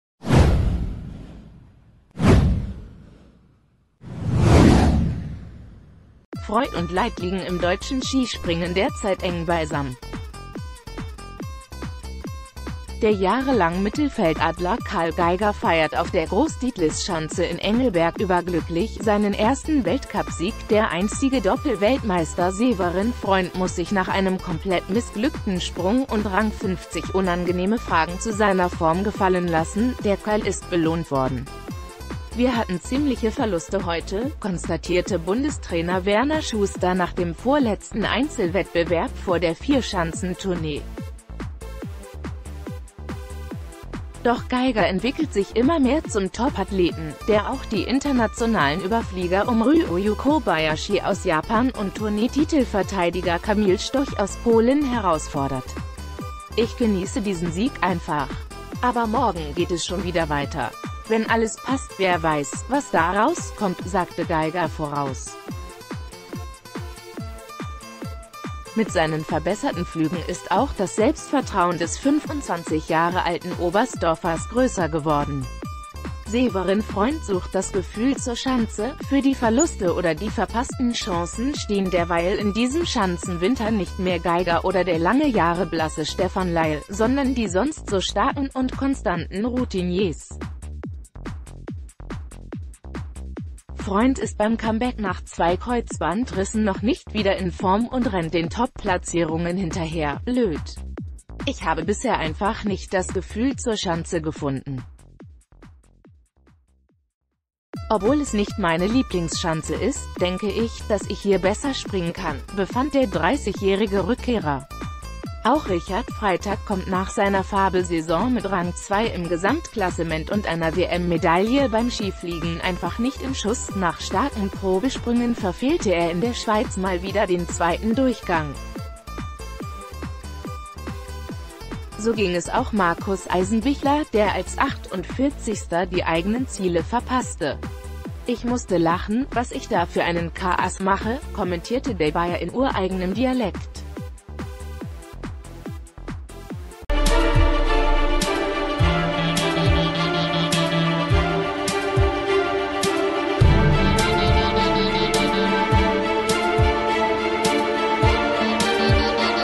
Skispringen: Karl Geiger jubelt, Severin Freund hadert - Freud und Leid beim DSV
Freud und Leid liegen im deutschen Skispringen derzeit eng beisammen. Der jahrelang Mittelfeld-Adler Karl Geiger feiert auf der Gross-Titlis-Schanze in Engelberg "überglücklich" seinen ersten Weltcup-Sieg, der einstige Doppel-Weltmeister Severin Freund muss sich nach einem komplett missglückten Sprung und Rang 50 unangenehme Fragen zu seiner Form gefallen lassen."Der Karl ist belohnt worden. Wir hatten ziemliche Verluste heute", konstatierte Bundestrainer Werner Schuster nach dem vorletzten Einz...